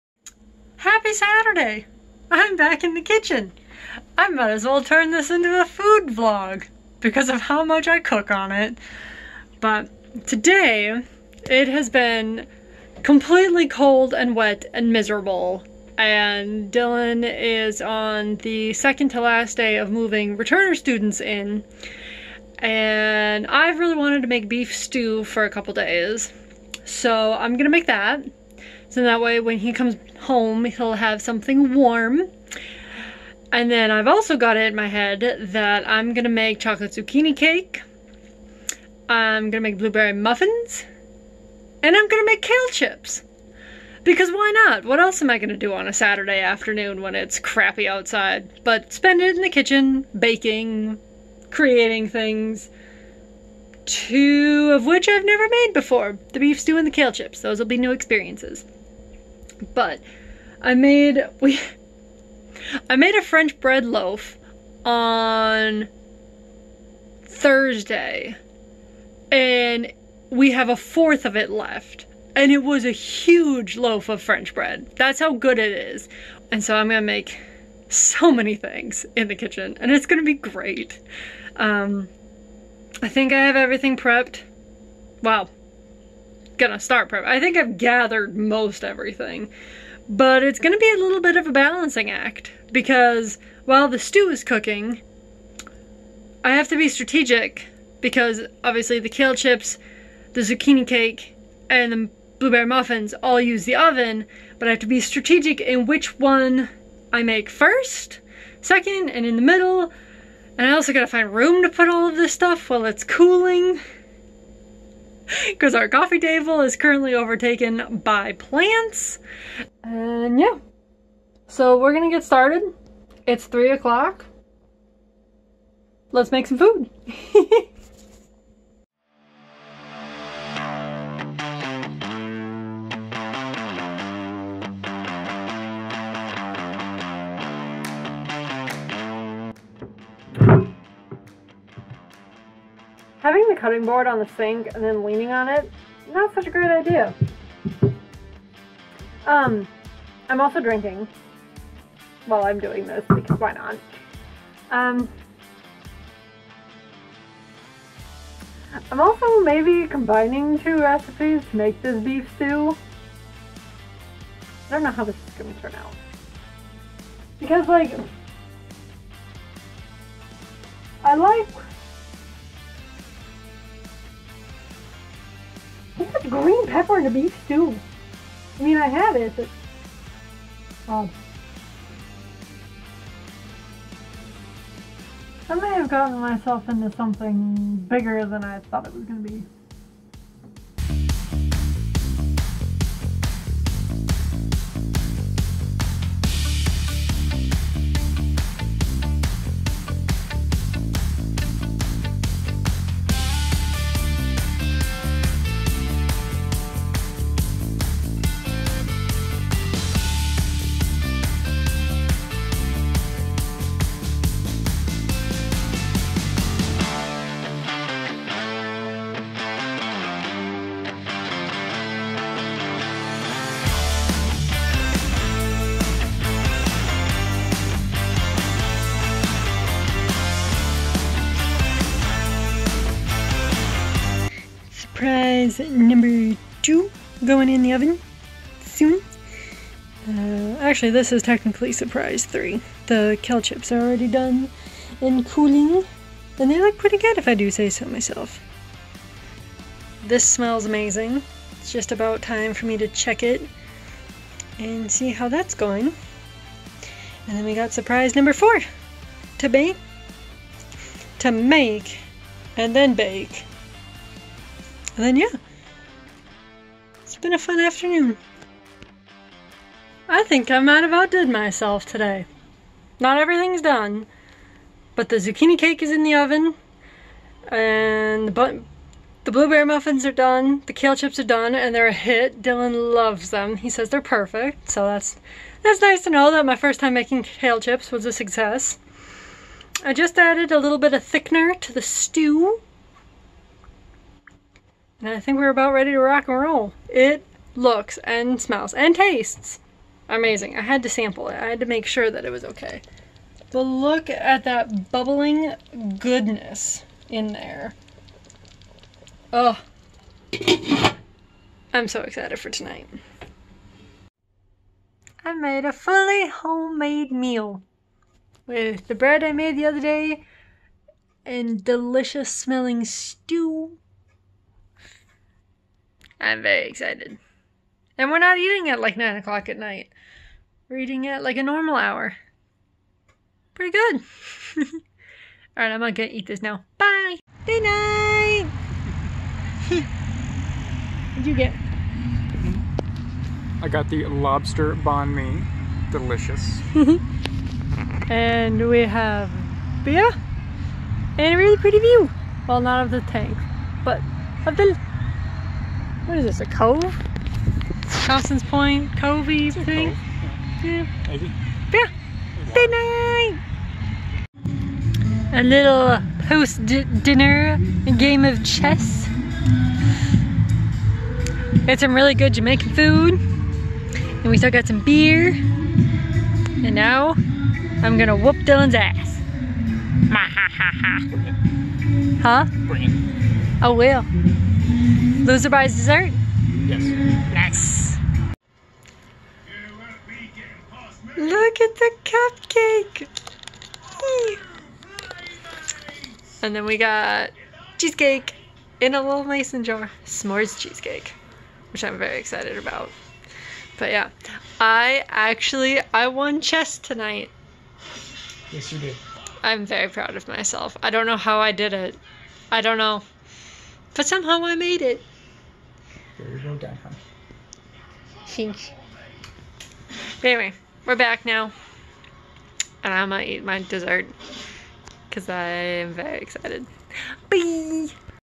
Surprises & Date Night
I spent an afternoon in the kitchen and we went on a date night picnic at Thompson's Point!

A Minnesota girl living in a Maine world! Come along as I vlog my fairly normal/sometimes exciting adventures :)